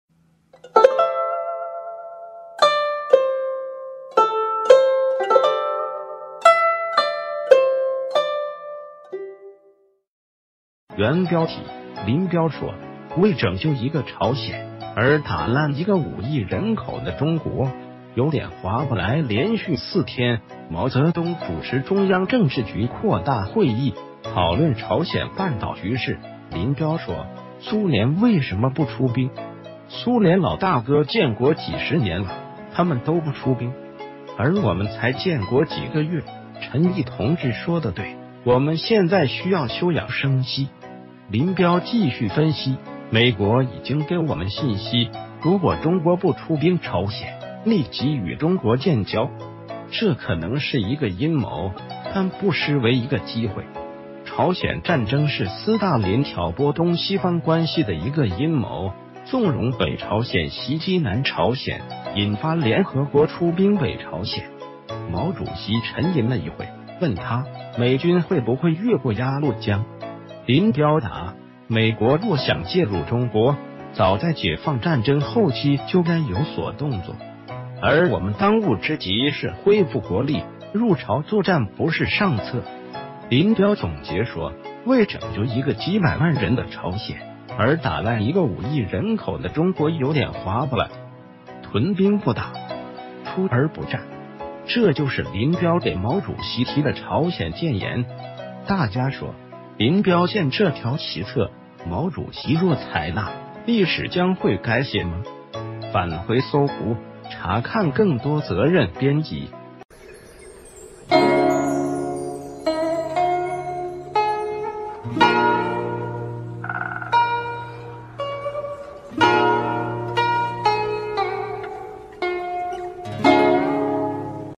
林彪說：為拯救一個朝鮮，而打爛一個五億人口的中國有點劃不來_毛主席
原標題：林彪說：為拯...